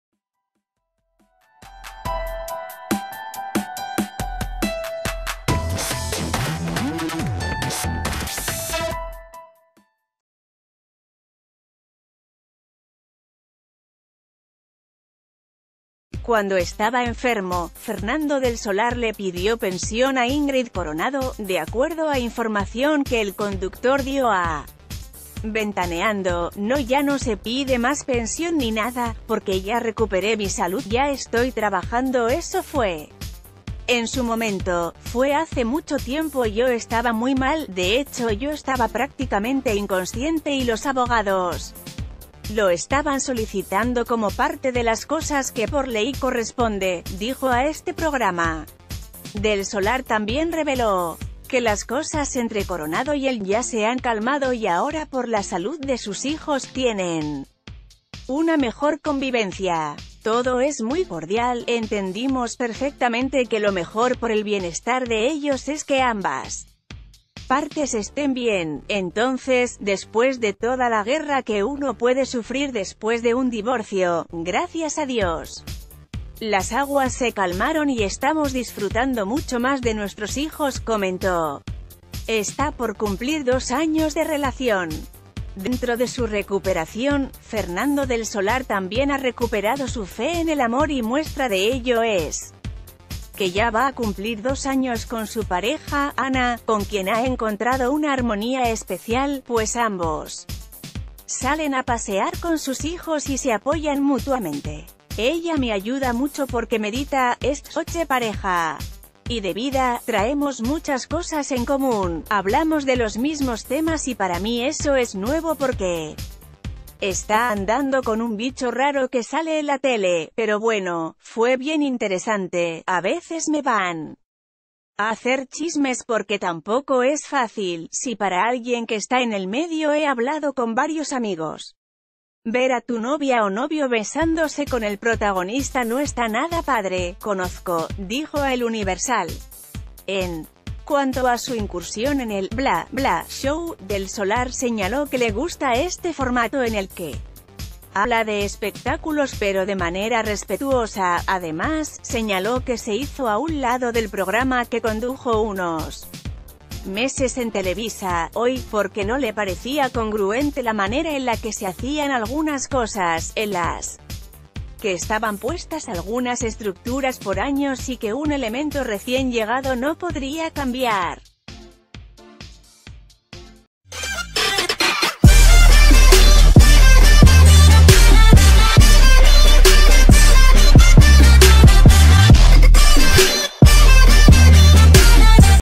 Fernando del Solar pidió pensión a Ingrid Coronado
Cuando estaba enfermo, Fernando del Solar le pidió pensión a Ingrid Coronado, de acuerdo a información que el conductor dio a 'Ventaneando'  "No ya no se pide más pensión ni nada, porque ya recuperé mi salud, ya estoy trabajando eso fue en su momento, fue hace mucho tiempo yo estaba muy mal, de hech...